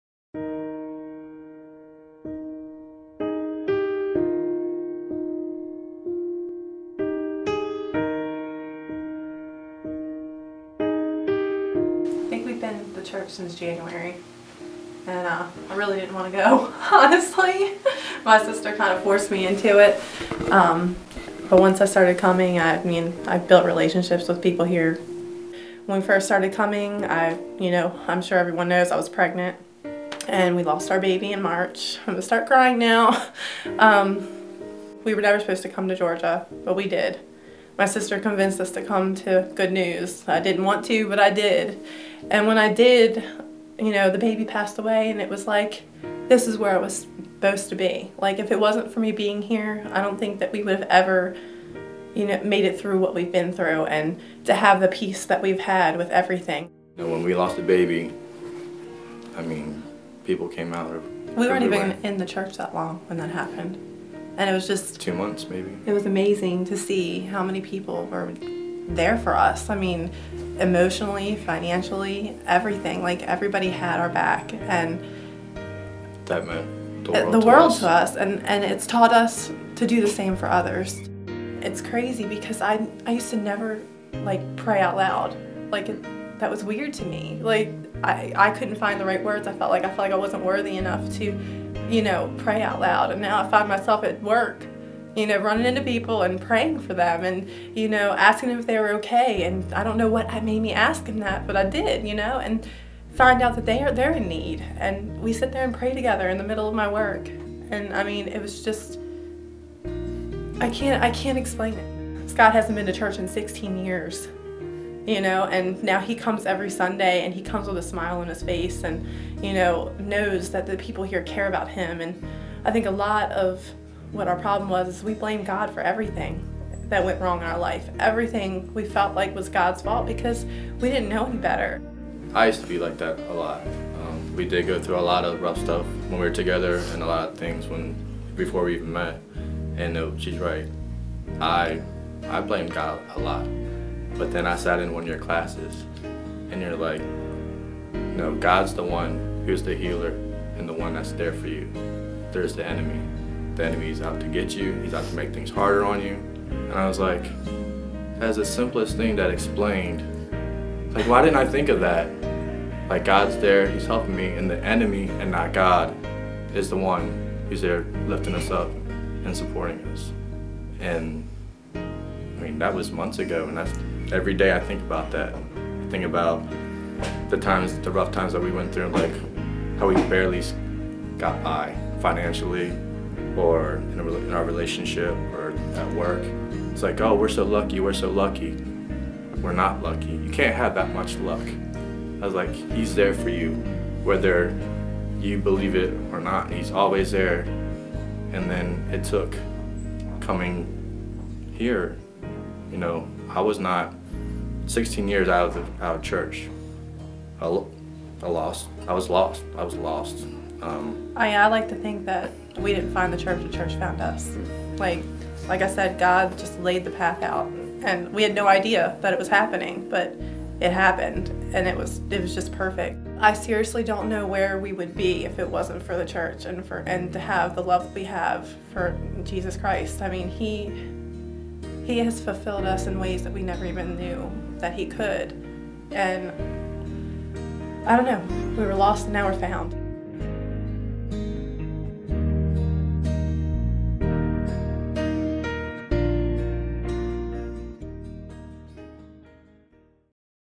Scott & Amanda Tyler's Life-Giving Testimony
Scott & Amanda have been impacted by Good News Church far more than they ever thought possible.  Their story is one of tragic loss and extreme hope.  They found that God has kept them through it all and they trust Him as the good Father that He is.